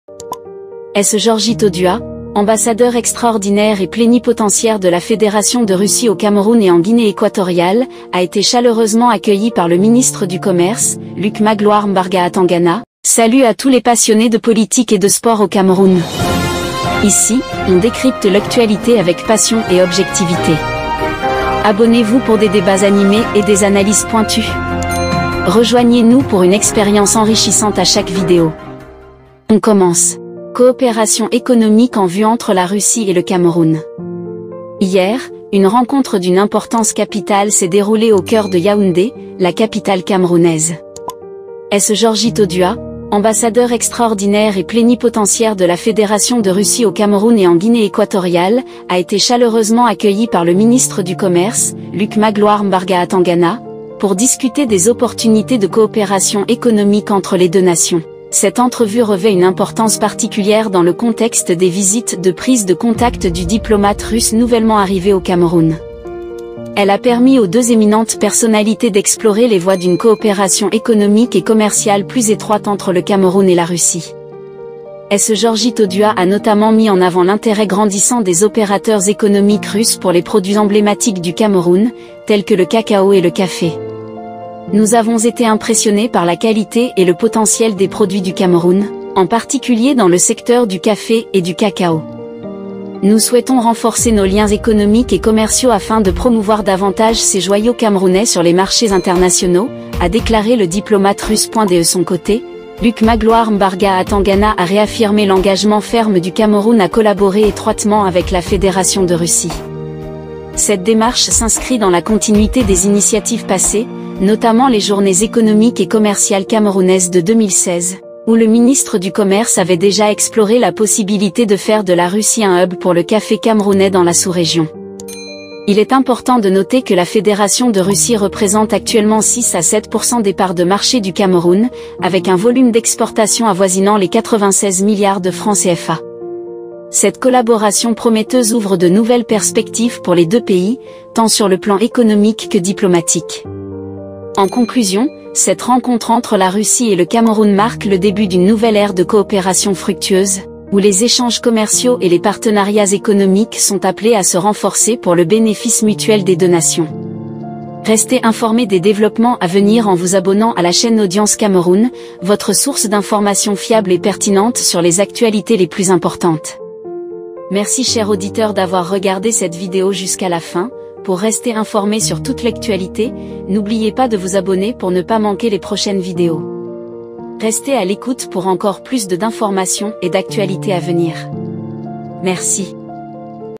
QUI AURAIT CRUE !!! Coopération économique : la Russie s’intéresse au Cacao et Café camerounais.
Description de la vidéo : Découvrez les coulisses d'une rencontre historique entre la Russie et le Cameroun, où les discussions ont porté sur le renforcement des liens économiques et commerciaux, notamment dans les secteurs du cacao et du café. Dans cette vidéo captivante, plongez au cœur des échanges entre S.E. Georgy Todua, Ambassadeur Extraordinaire et Plénipotentiaire de la Fédération de Russie au Cameroun et en Guinée Équatoriale, et Luc Magloire Mbarga Atangana, ministre du Commerce du Cameroun. Découvrez les opportunités économiques en jeu et les perspectives prometteuses pour ces deux nations. Abonnez-vous dès maintenant à la chaîne Audience Cameroun pour rester informé des actualités les plus importantes du pays et ne manquez aucune mise à jour sur cette coopération économique en plein essor.